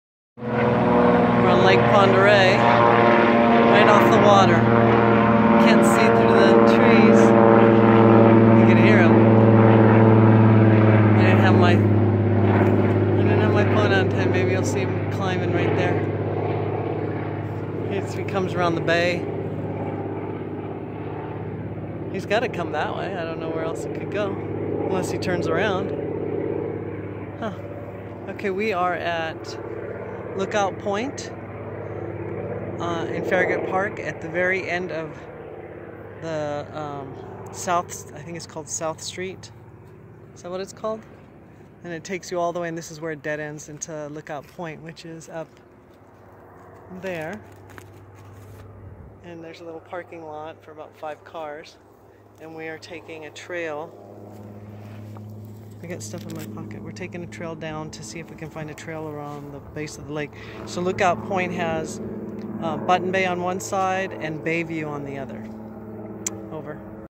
Plane taking off from water on Lake Pend Oreille from Lookout Point in Farragut Park
Looking out over Lookout Point and a plane took off from the water below.